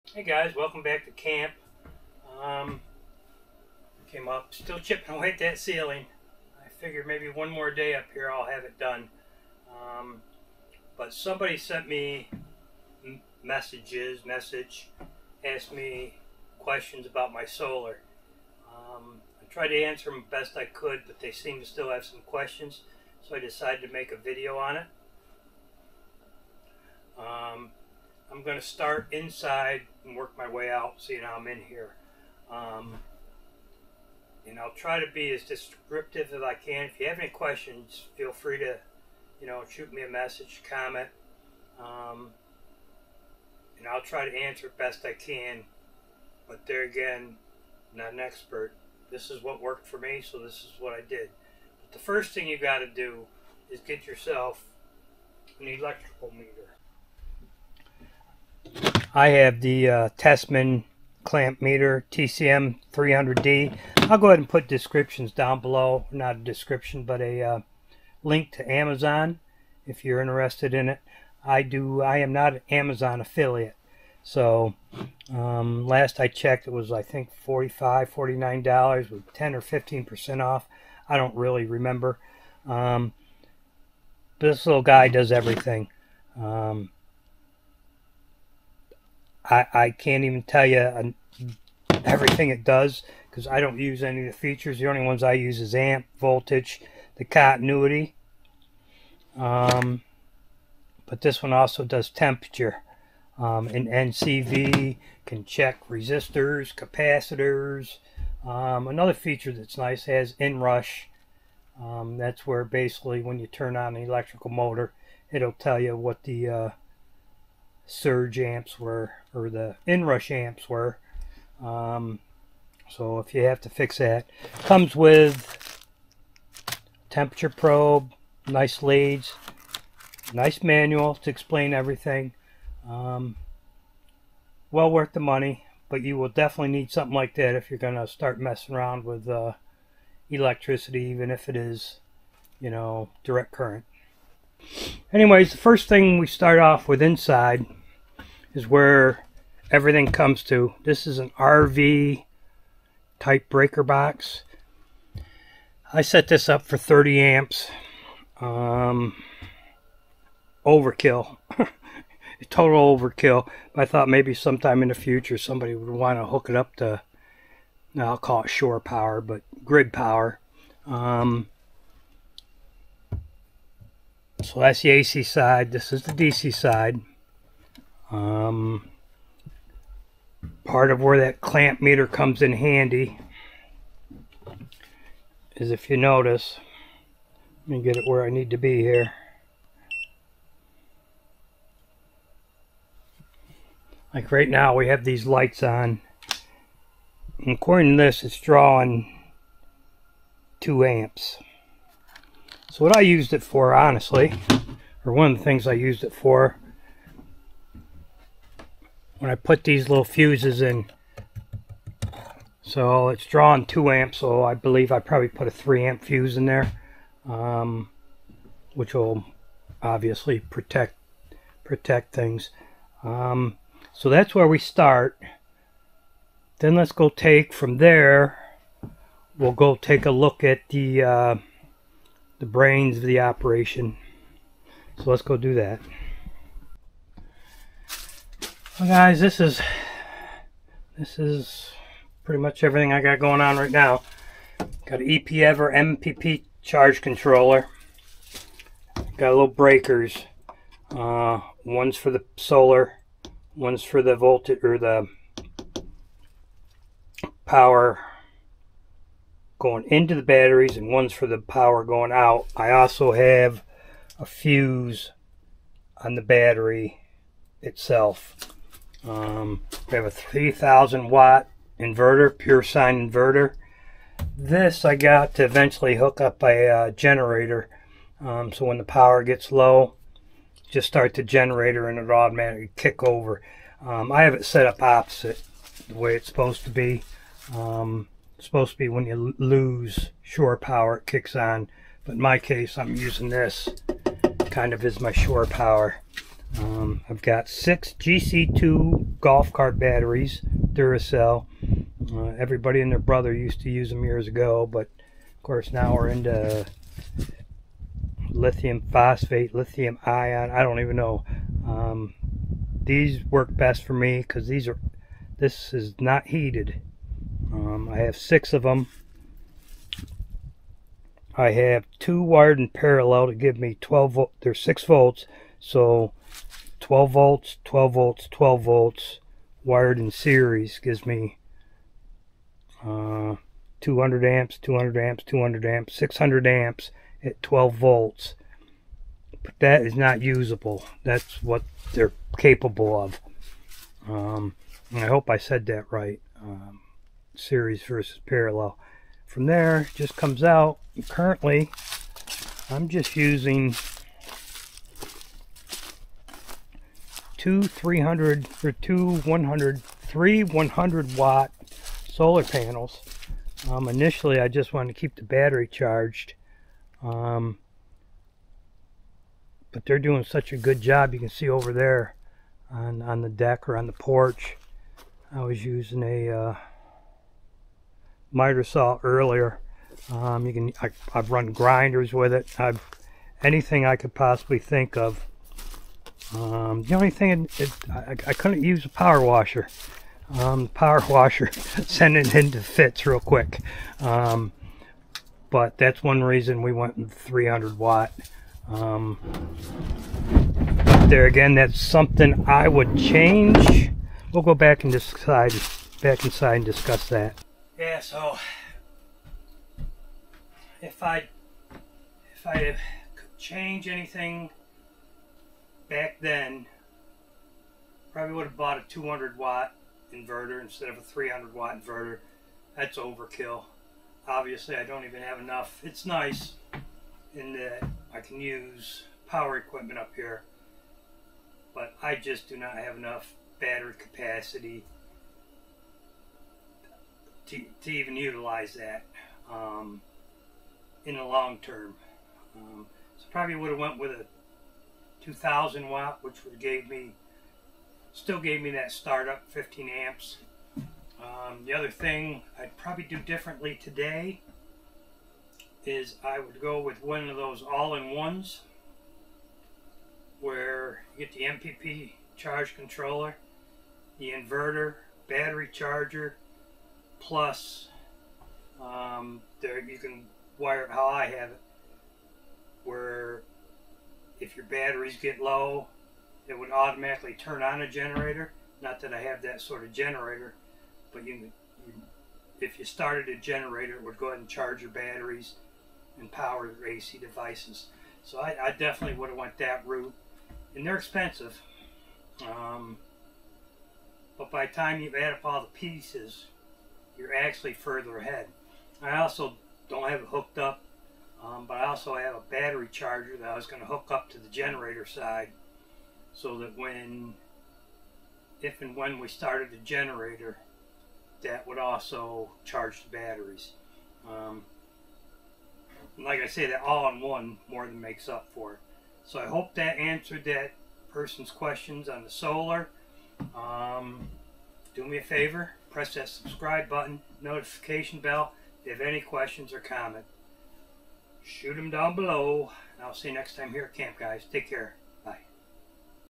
Powering camp with solar: A complete walkthrough.
Tried to answer questions I get about my solar.
Hope this video was some help..
Some of the items seen in this video.

TESMEN Smart Meter 300D
EPEVER MPP
Renogy 3000W pure Sine inverter
12 volt fans
fan covers
12volt to usb

off grid